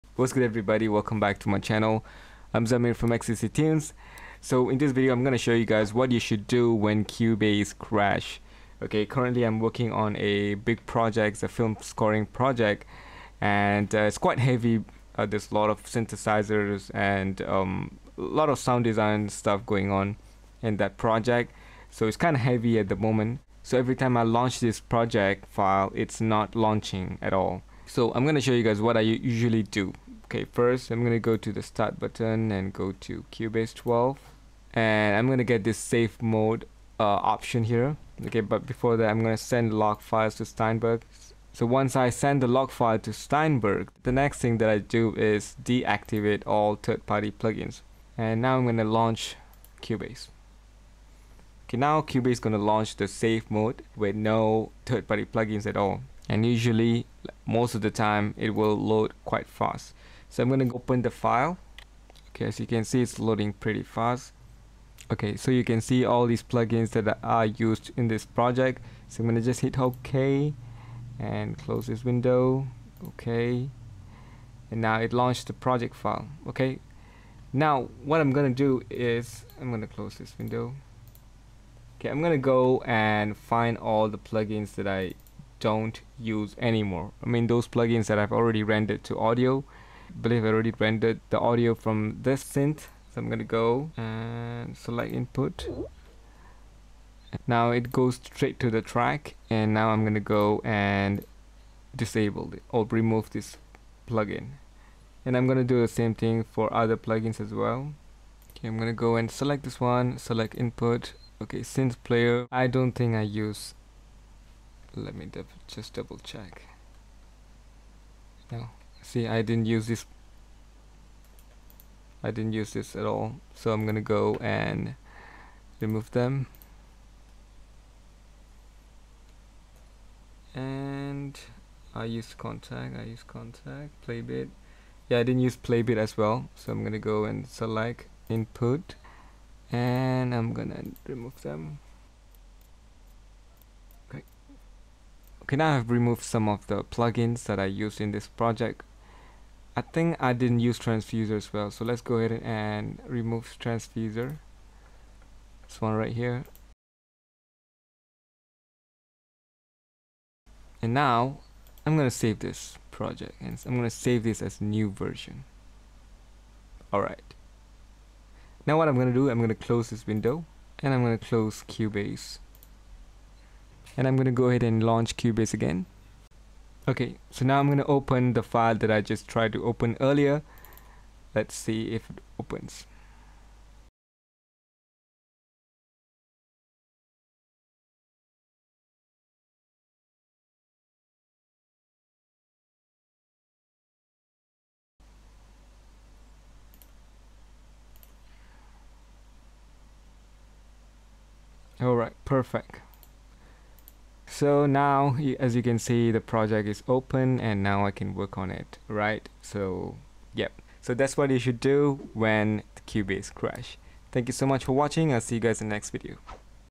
CUBASE QUICK TIP | WHAT YOU SHOULD DO WHEN CUBASE CRASH
In this video, I'll show you guys what to do when Cubase crash.